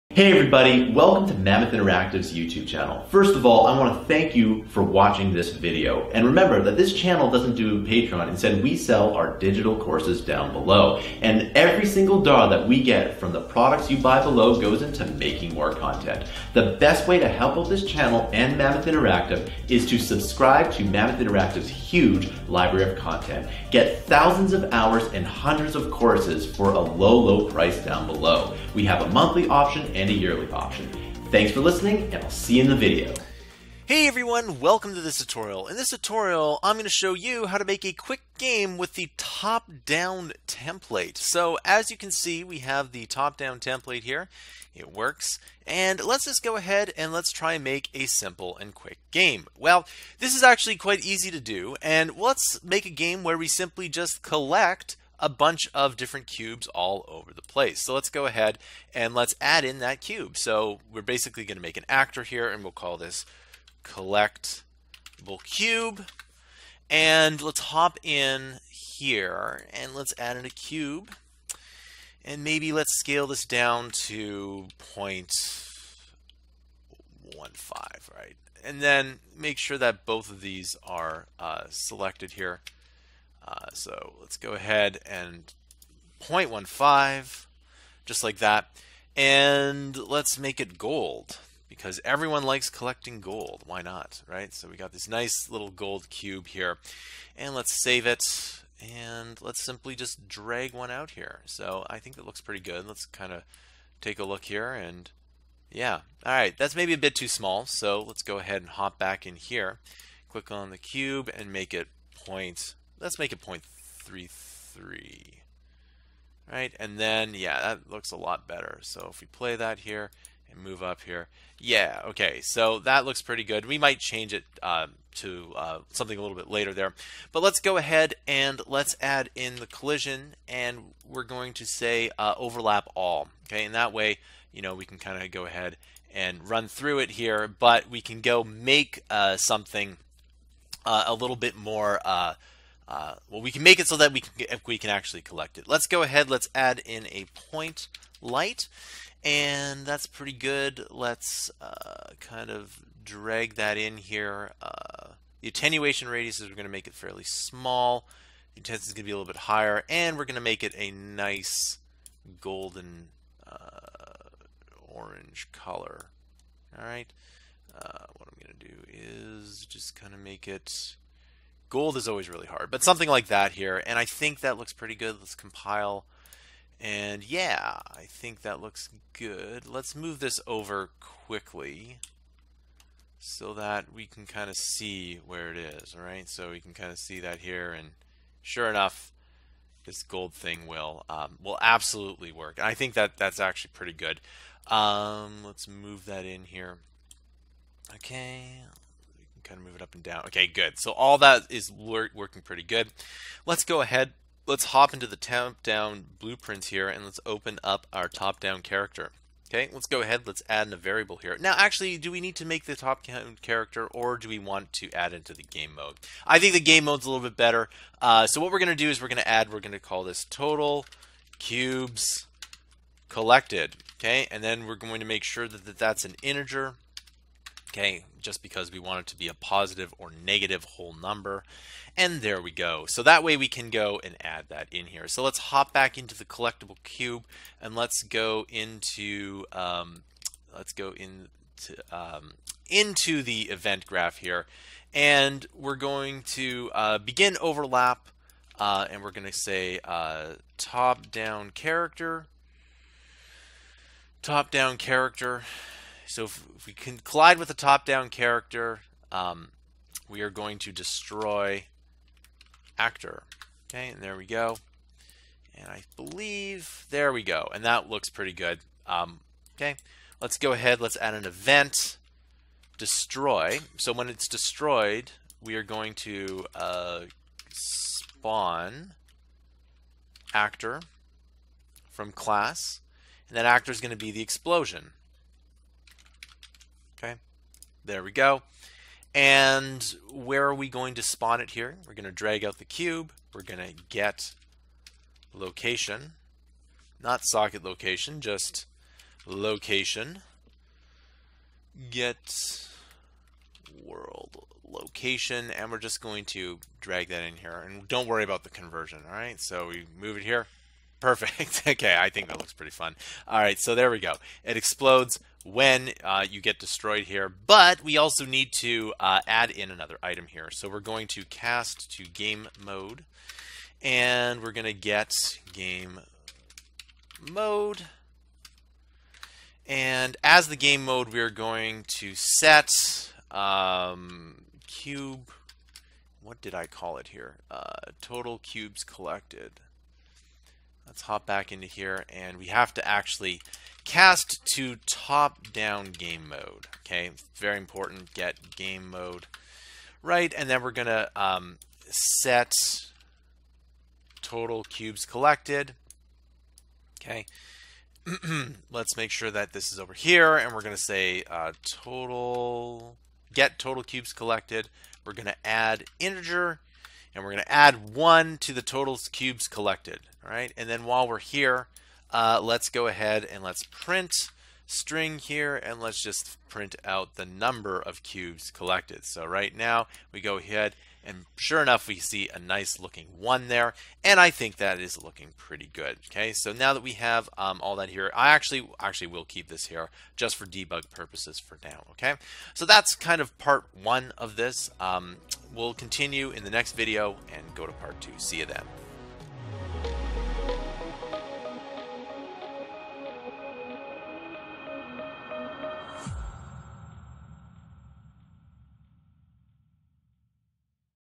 How To Make A Game In Unreal Engine with Top Down Template | Part 1
Learn How to Make a Game in Unreal Engine with a top down template in 15 minutes.

In this video, I will explain how you can make games with top down template in Unreal Engine in 15 minutes.